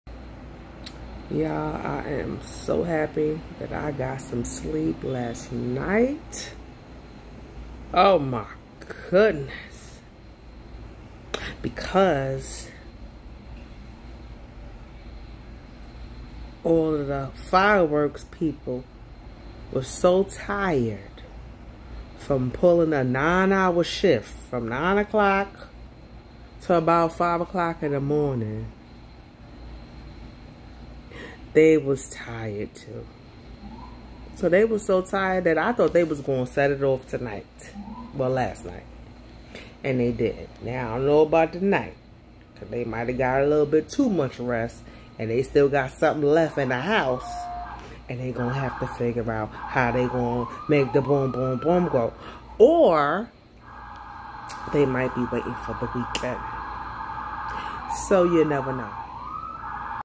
Finally Got Some Sleep. July 6, 2021
The Fireworks people went to bed last night FINALLY!!